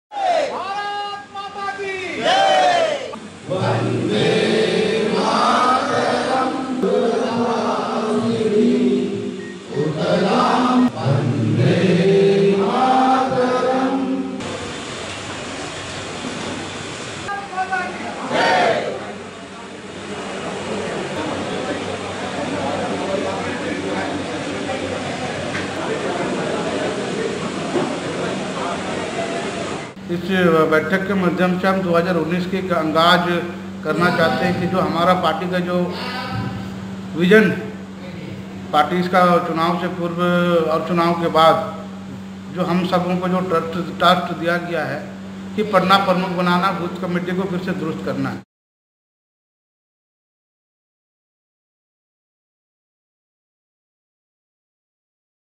सरायकेला : भाजपा कार्यकर्ता सम्मेलन में महिला मोर्चा की जिलाध्यक्ष को नहीं मिला मंच पर स्थान
मिशन 2019 की तैयारियों में भाजपा पूरी तरह से जुट गई है। सरायकेला विधानसभा स्तरीय कार्यकर्ता सम्मेलन में ऐसा ही नजारा देखा गया। सरायकेला सामुदायिक भवन परिसर में आज विधानसभा स्तरीय कार्यकर्ता सम्मेलन का आयोजन किया गया।